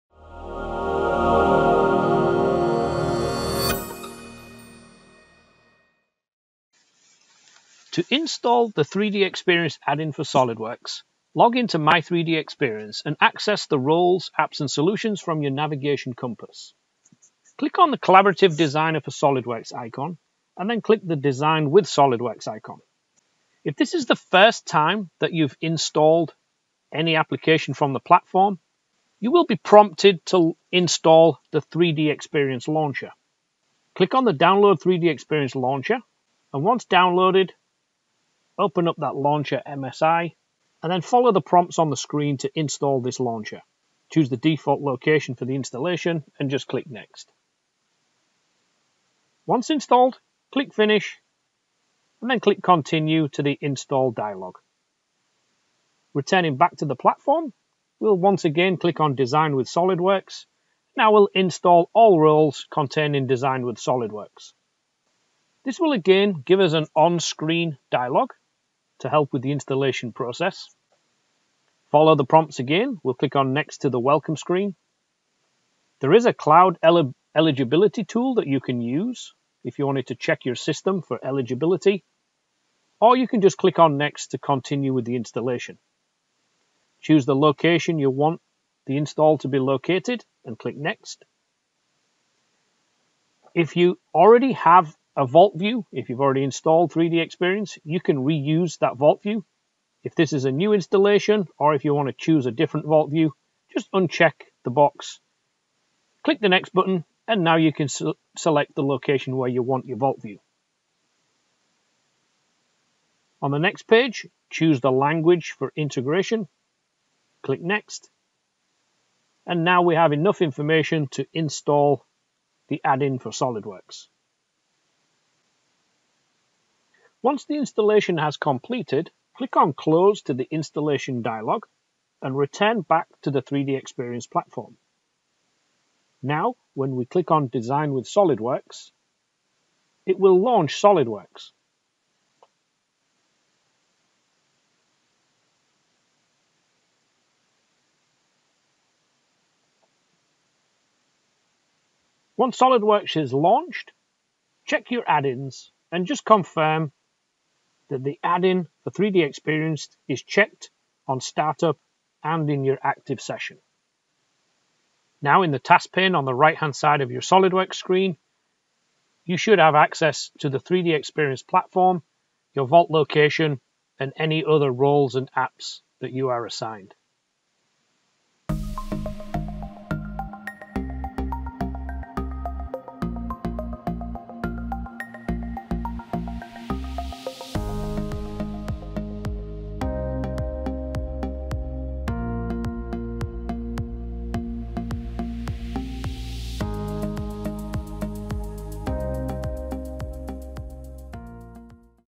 How to Install the 3DEXPERIENCE Add-in for SOLIDWORKS
Learn more about how the 3DEXPERIENCE Platform can help your business

The 3DEXPERIENCE Platform can expand the capabilities of @solidworks Desktop products with the 3DEXPERIENCE Add-in.  and login to gain access to your files and software.